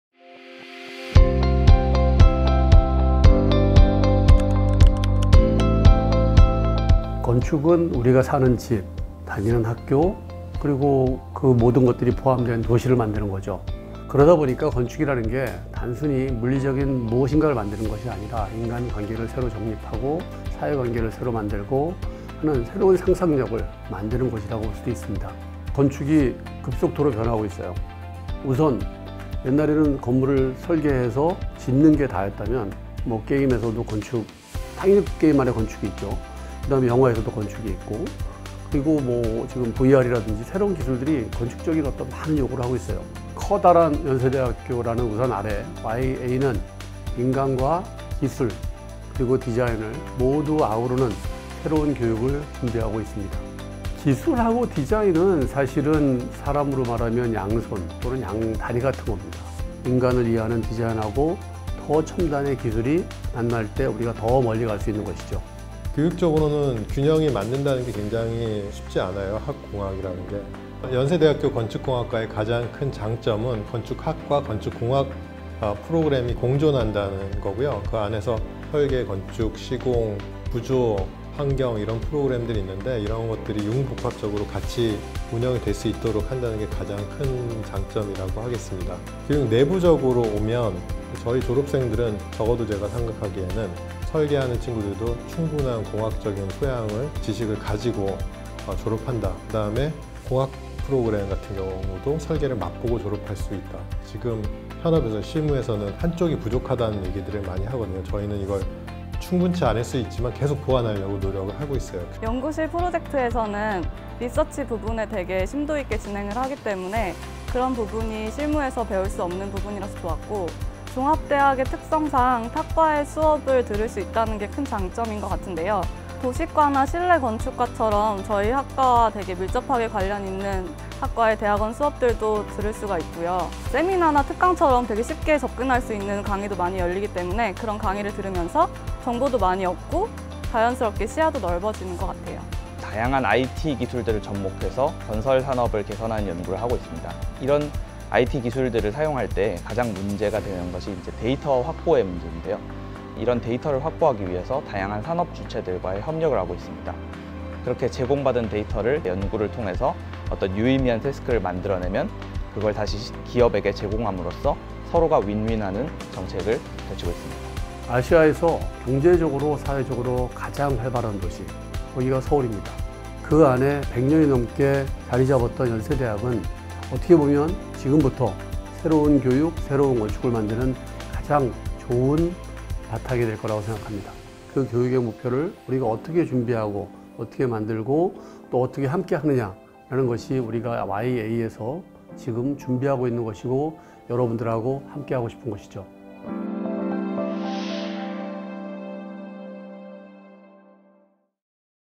Yonsei Architecture and Architectural Engineering
[연세대학교 건축공학과]

The most economically and socially alive city in Asia, is Seoul, where we are right now. Within such a city, Yonsei University, which has held its ground for over a century, might be the best setting for a new learning and a new architecture.

I Film produced in 2021